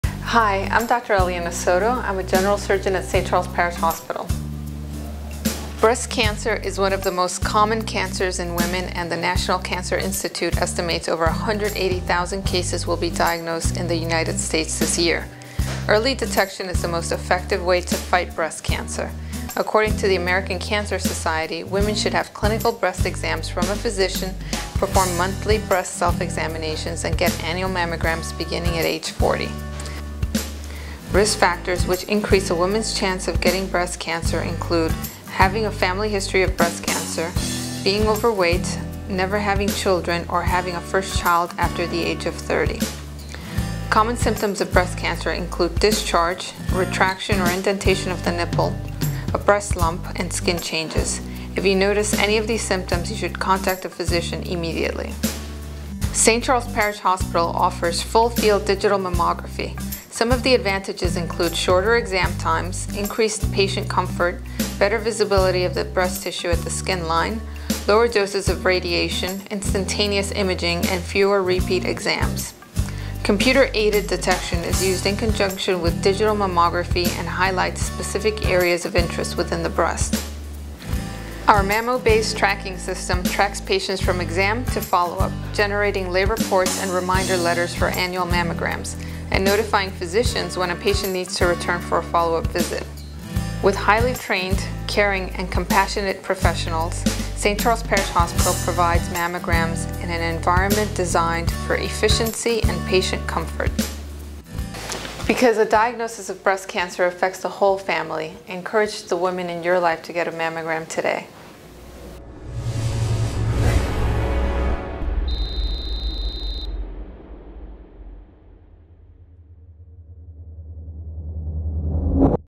St. Charles Parish Hospital / Breast Cancer Awareness
St. Charles Parish Hospital is an accredited digital mammography facility and offers a comprehensive breast imaging program utilizing Full Field Digital Mammography.  Some of the advantages include shorter exam times, increased patient comfort, better visibility of the breast tissue at the skin line, lower doses of radiation, instantaneous imaging and fewer repeat exams.  Computer Aided Detection is used in conjunction with digital mammography and highlights specific areas of interest within the breast.